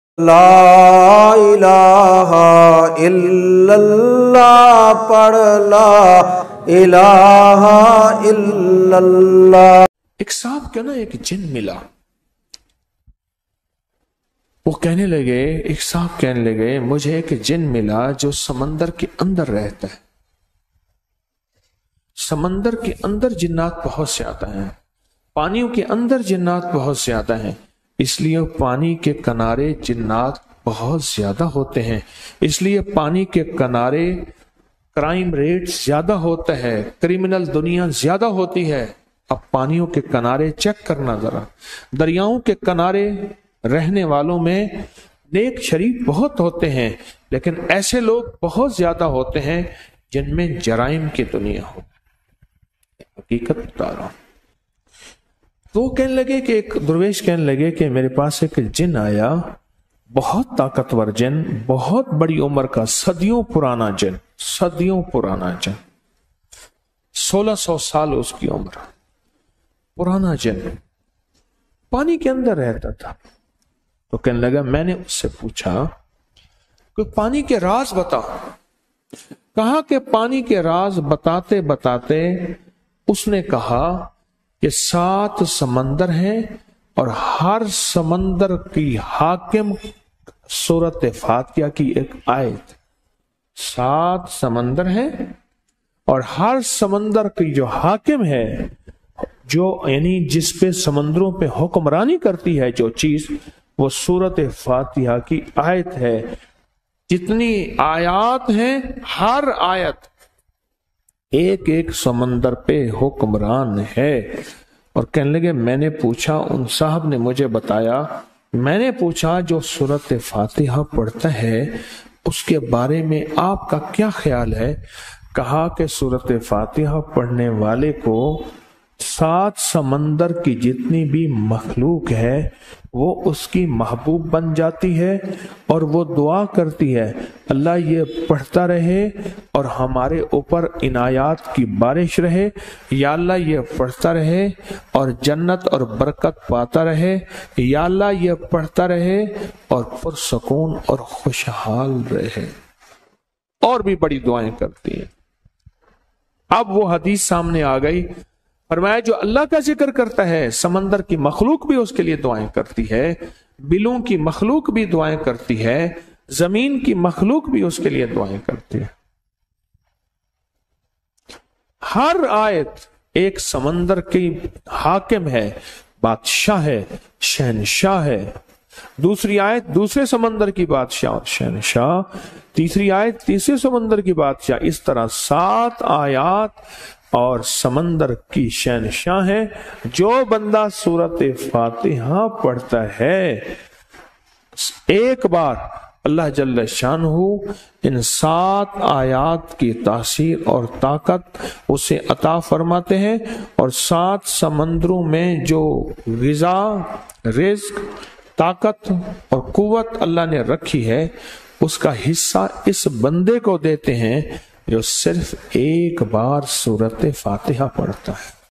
7 Ayaat or 7 Sumandar Kay Khazanay | Treasure Keys in Quranic Ayaat
7 Ayaat or 7 Sumandar Kay Khazanay | Treasure Keys in Quranic Ayaat

Sheikh-Ul-Wazaif' Hazrat Maulana Zafar iqbal Nafees